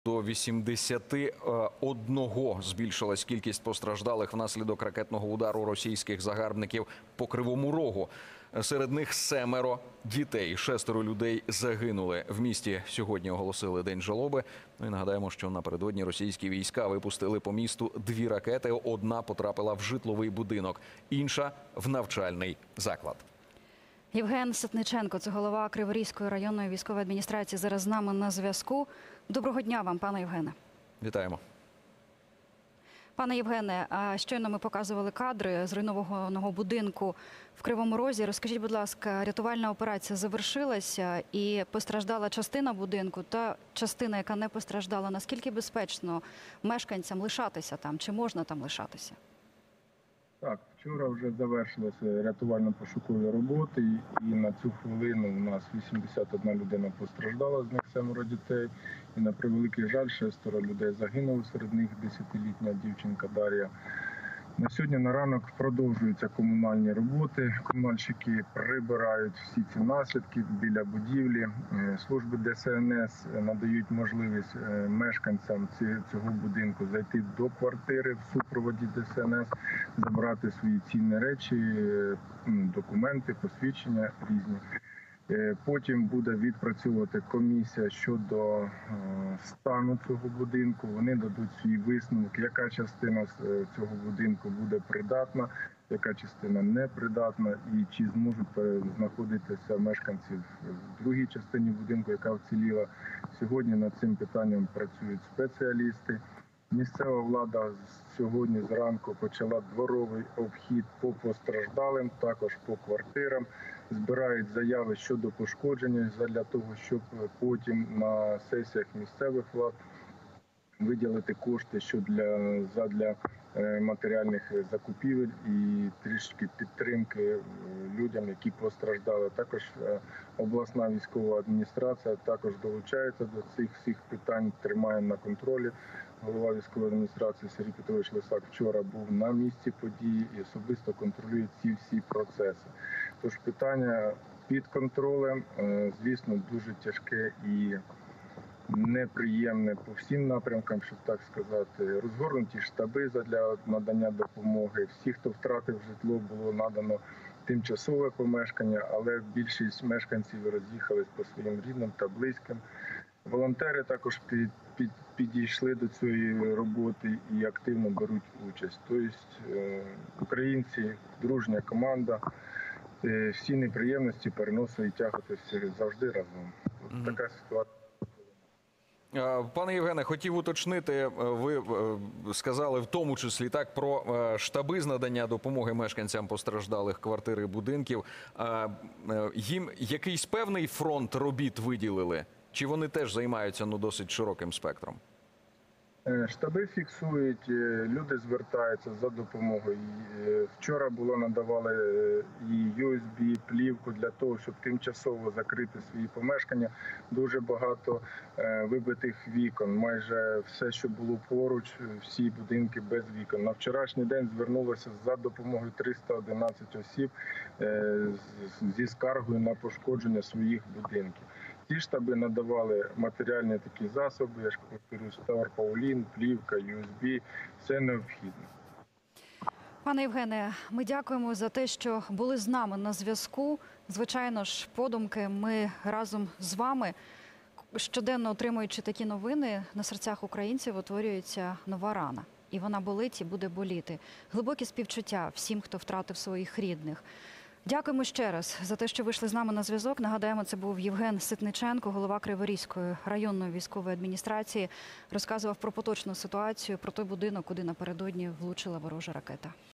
❗ Наслідки удару по Кривому Рогу: 81 людина постраждала, 6 жителів будинку - загинули. СИТНИЧЕНКО
Євген Ситниченко, начальник Криворізької районної військової адміністрації розповів останні подробиці щодо обстрілів Кривого Рогу.